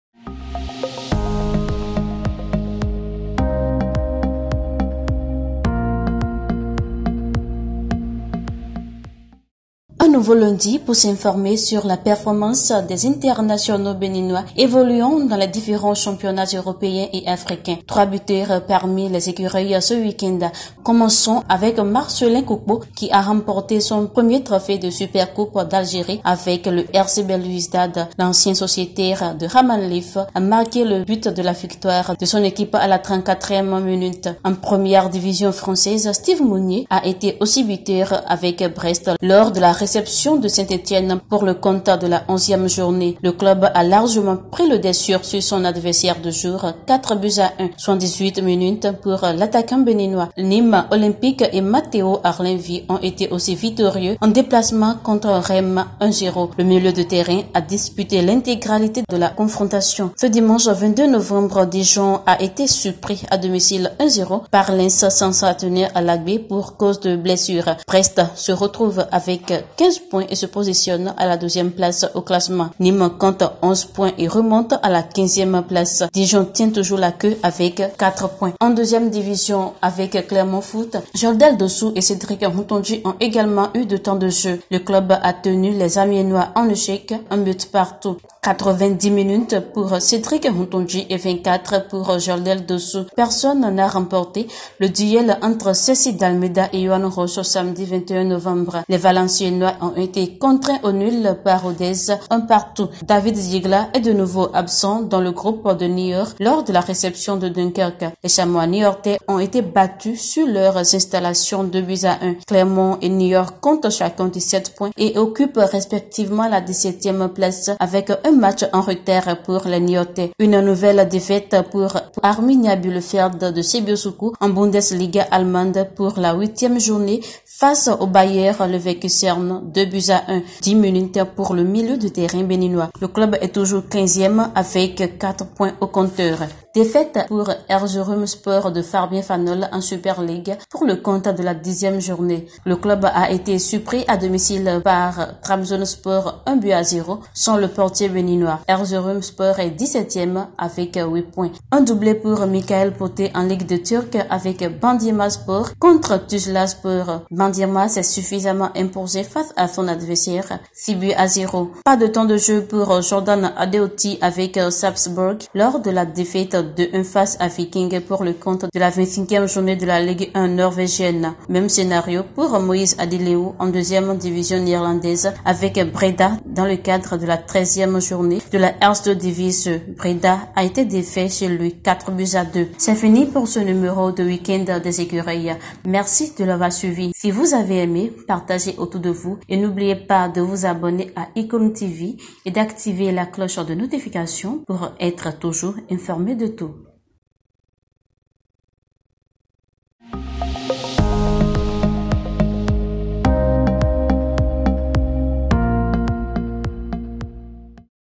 Weekend des Ecureuils : Poté, Mounié buteurs, Koukpo gagne une coupe en Algérie
Trois buteurs parmi les internationaux béninois ce weekend. Marcelin a marqué son deuxième but avec le CR Belouizdad et son tout premier en match officiel avec le club. Une réalisation qui permet à l'équipe de remporter la Super coupe d'Algérie. Un doublé pour Michael Poté avec Bandirmaspor. Steeve Mounié a été aussi buteur.

 Suivez ici le point de la prestation des internationaux béninois.